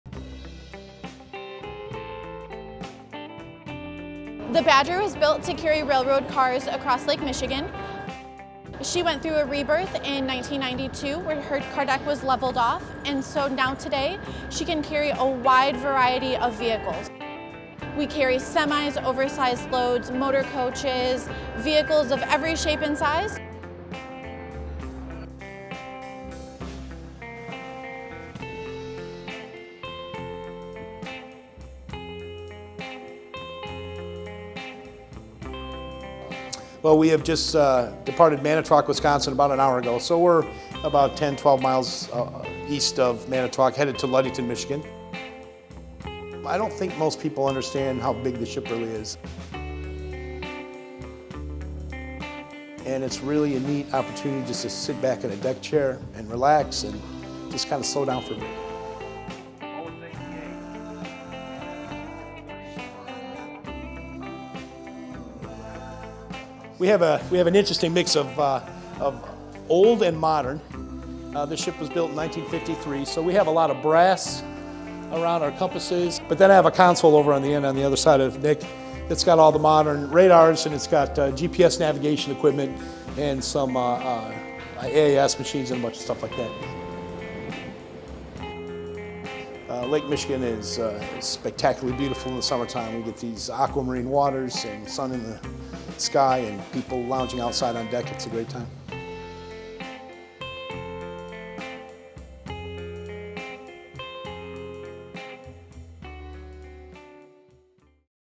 Cruise across Lake Michigan on the Badger car ferry
The S.S. Badger car ferry based in Ludington, Michigan makes the trip across to Manitowoc, Wisconsin and back daily. The 410-foot ferry was built in 1952 and is now a National Historic Landmark. The Badger can carry 600 passengers and 180 automobiles in a single trip across the lake and does it in about 4 hours.

MLive video: Cory Morse, Garret Ellison, Neil Blake